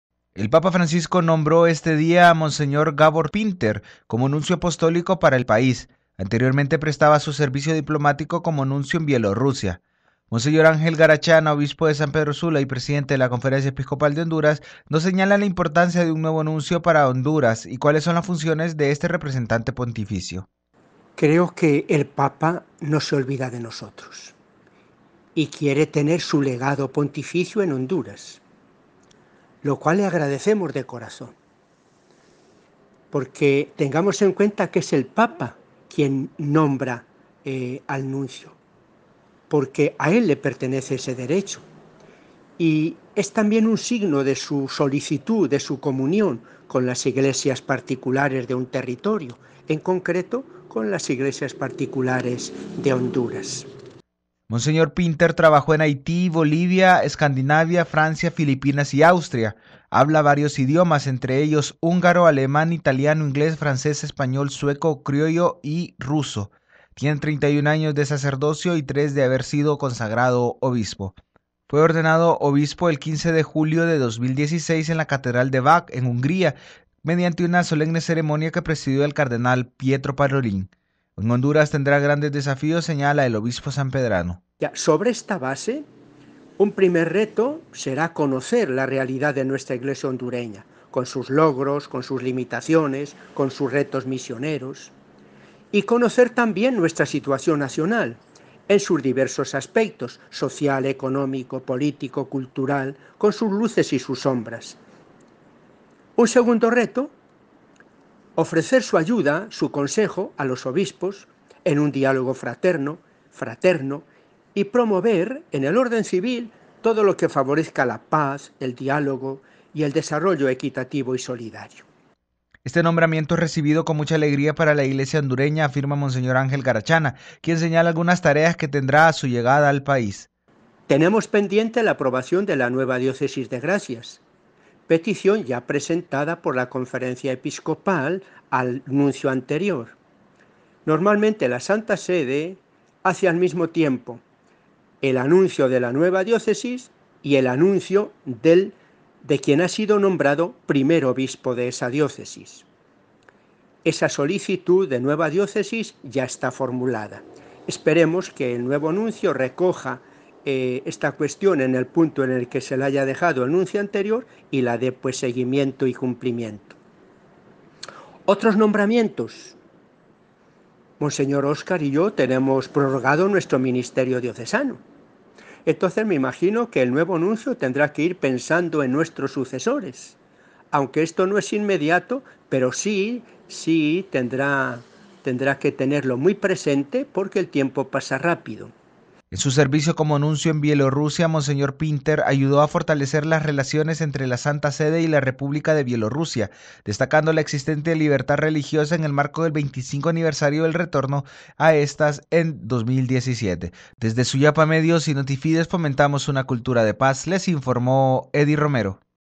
Nuncio Apostólico para Honduras
La Santa Sede ha nombrado ya un Nuncio Apostólico para Honduras. En el siguiente informe, le detallamos quién es el representante del Papa ante el gobierno y Estado hondureño.